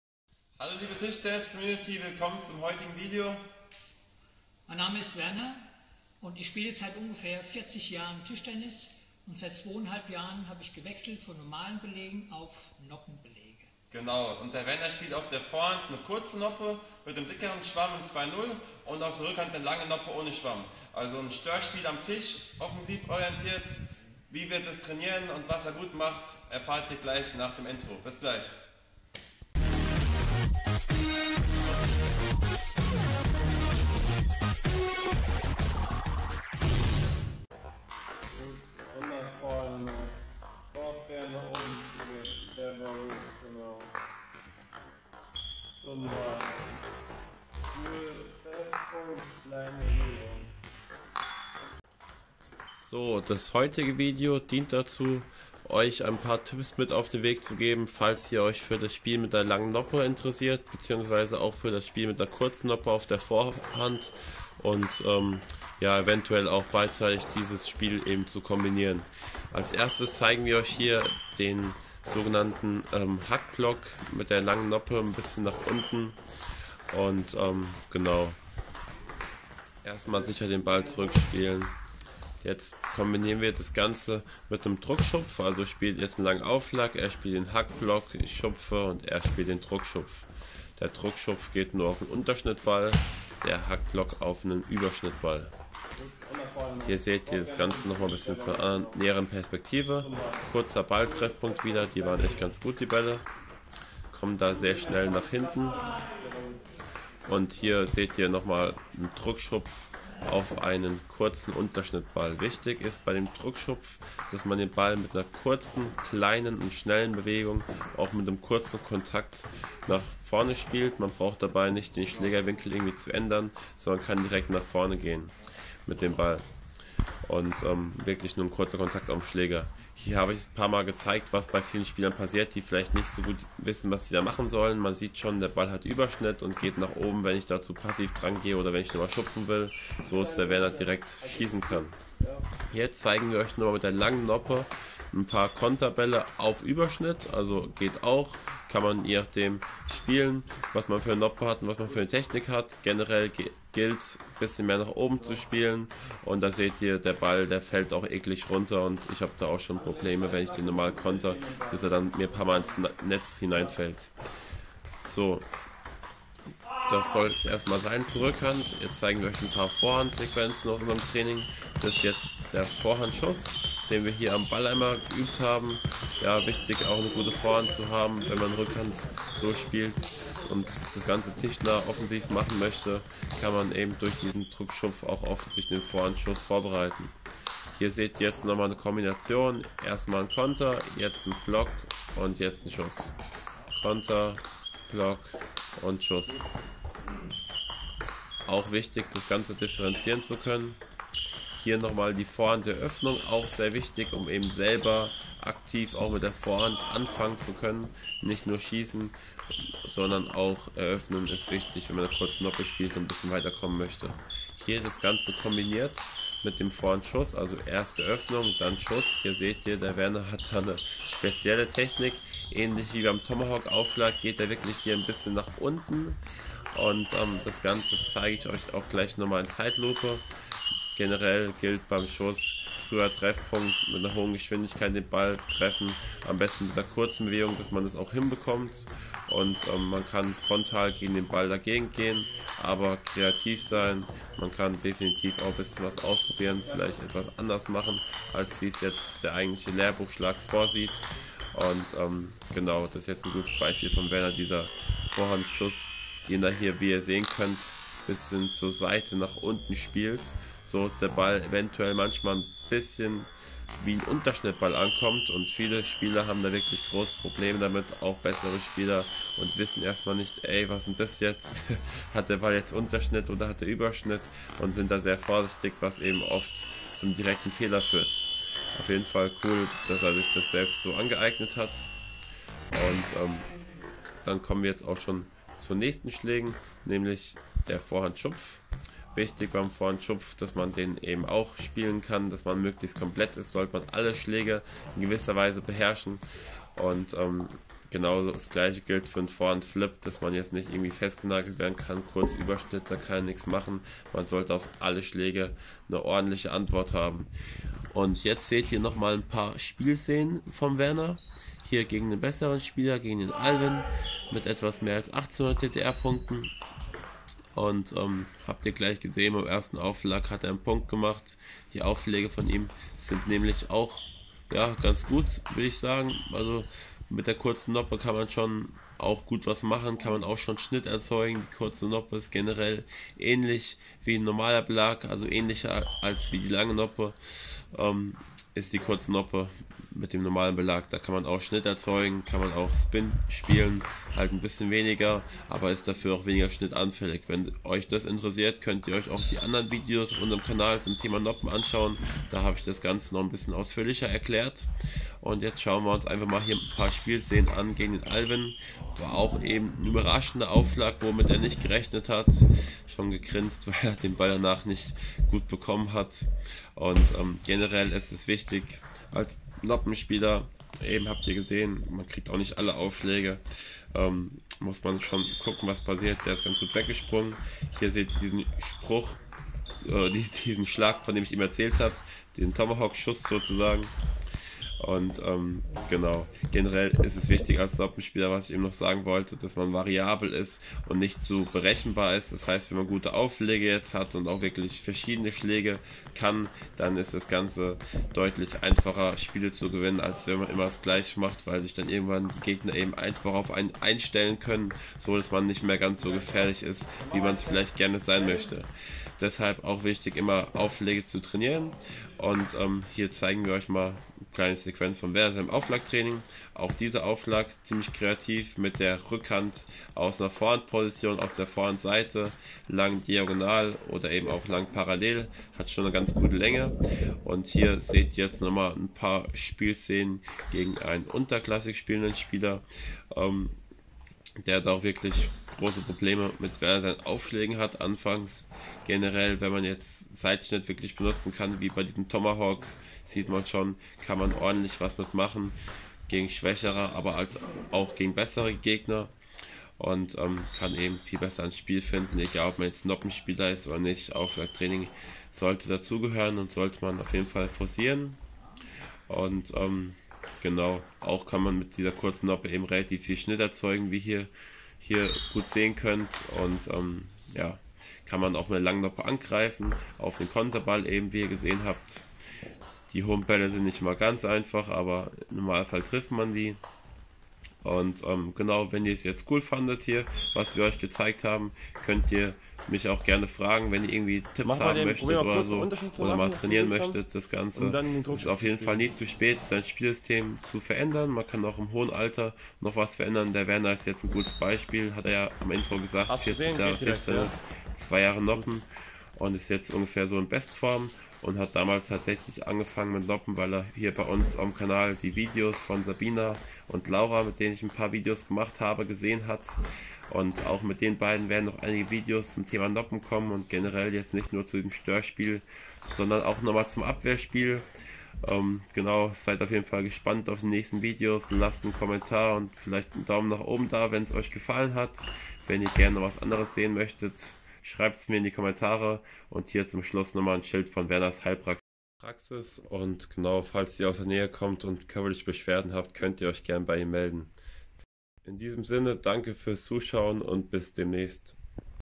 Nach 40 Jahren Tischtennis auf Noppen umsteigen ?!
Werner ist nach 40 Jahren Tischtennis mit noppen innen Belägen auf Noppen außen Beläge umgestiegen und nach einigen Trainingseinheiten in den letzten Monaten aktuell wohl stärker denn je.🏓🔥
Jeder sollte verschiedene Spielweisen ausprobieren bevor er sich auf einen Spielstil festgelegt, denn gerade diese große Vielfalt macht unseren Sport aus.👍
Im Video einige Tipps zum tischnahen Störspiel ( mit langer Noppe).
Viel Spaß mit diesem Video,  die nächsten Wochen kommen noch einige andere Videos der letzten Wochen und auch neue Aufnahmen. 😃👍